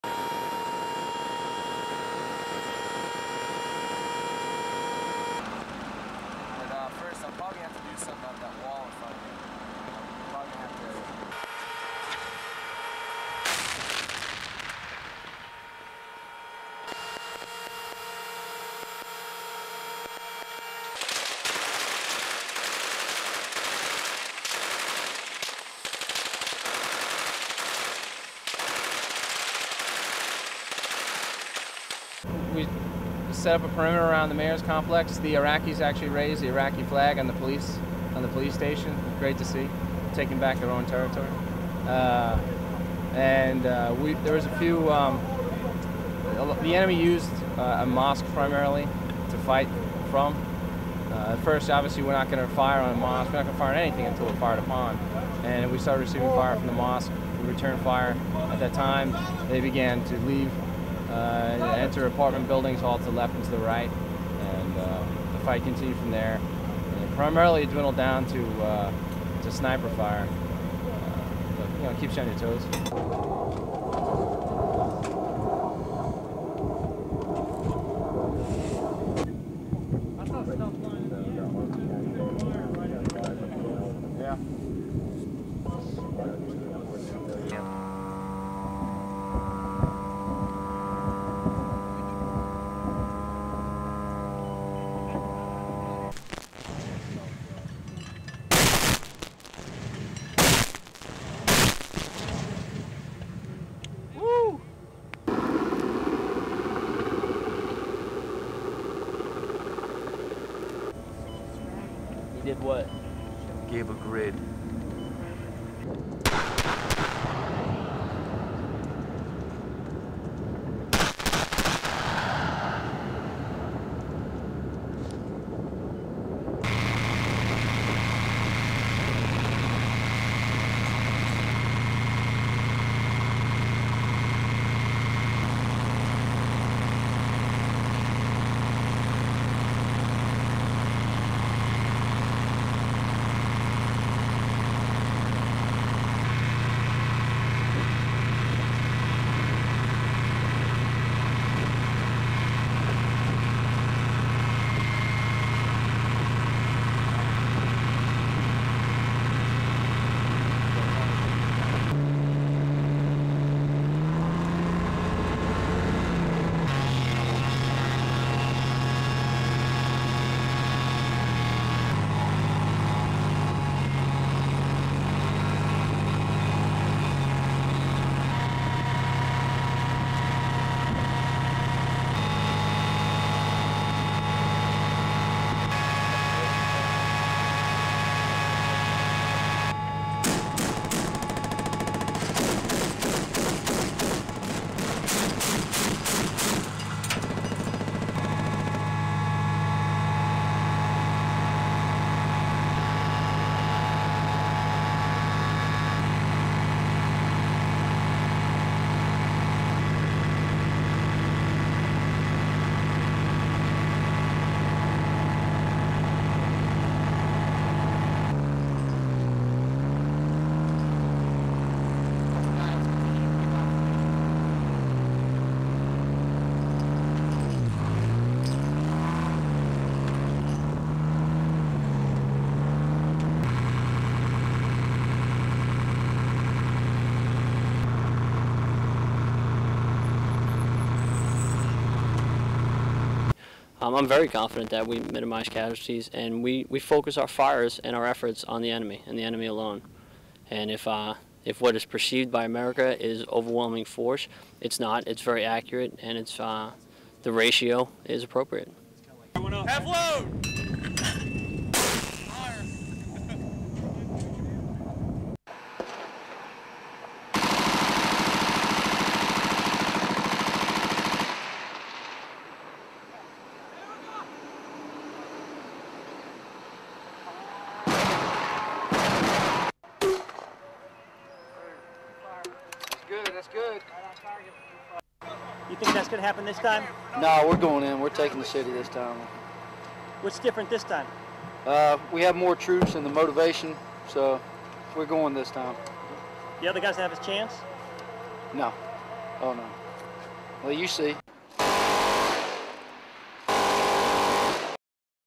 WRAP Pictures of fighting in streets of Fallujah
November 10, 2004 ++Day shots++
1. View from armoured vehicle pointing at burning building inside Fallujah
2. Pull out from Saddam-era Iraqi flag over central Fallujah police station
3. Various UPSOUND gunfire as another US vehicle shoots at building
4. SOUNDBITE: (English) Corporeal Quaeir Taylor, US soldier
"We set a perimeter around the mayor's complex. The Iraqis actually raised the Iraqi flag at the police station. Great to see (them), taking back their own territory. There is a few... the enemy used a mosque primarily to fight from. First off we're not going to fire on a mosque, we're not going to fire on anything until we are fired upon, and we started fire from the mosque and returned fire and at that time they began to leave and enter apartment buildings from the left and the right, and fighting continued from there. Primarily it's all down to sniper fire, keeps you on your toes."

5. Various of explosions on outskirts of the southern edge of Fallujah
6. UPSOUND Gunfire as smoke billows from building
7. Various of US military on outskirts of the southern edge of Fallujah
8. Mortars being fired
9. Helicopter flying overhead
10. More explosions

++Nightscope vision++
11. UPSOUND Gunfire, as explosions take place on the horizon

12. US Tanks
13. US soldier looking through binocular
14. Various US tanks
15. Destroyed building
16. Various of Fallujah buildings
17. Various on board tank
18. Shell bursts in sky
19. US soldier looking through binocular
20. Fallujah buildings
21. UPSOUND gunfire from US troops
22. Smoke billowing from building
23. More US tanks
24. Various on board tank heading towards Fallujah
25. SOUNDBITE: (English) Captain Griffin (first name not given), US army:
"I'm very confident that we can minimise casualties. We focus our fire on the enemy and the enemy alone, and if what is perceived by America is overwhelming force, it's not, it's very accurate and the ratio is appropriate."
26. US troops firing mortar

27. UPSOUND Gunfire as US soldiers run to position when offensive began
28. Silhouette of US soldier firing
29. SOUNDBITE (English) US soldier (name not given):
"No, we're going in, we're taking the city this time (Q: What's different this time?) We have more troops and the motivation, so we're going this time. No, you''ll see."
30. US troops firing from armoured vehicles

STORYLINE

Iraqi troops fought off militants and rebel snipers to raise the new Iraqi flag over central Fallujah's police station on Wednesday, as the US military said its swift advance had seen it seize control of 70 per cent of the militant stronghold.

Before dawn on Wednesday, US and Iraqi forces fought rebels to retake Fallujah's City Hall complex, which houses a police station and the mayor's office.

The Iraqi troops raised the flag after fending off rebels who fired from mosque minarets and nearby apartment buildings.

The US military said its troops had also secured several mosques used as militant command centres, key bridges and military building. The military said US troops pushed militants into a section of the city flanking the main east-west highway that bisects the rebel stronghold.

At least 71 militants had been killed by the beginning of the third day of intense urban combat, the US military said, with the casualty figure expected to rise sharply once Iraqis and foreign fighters killed in air strikes are accounted for.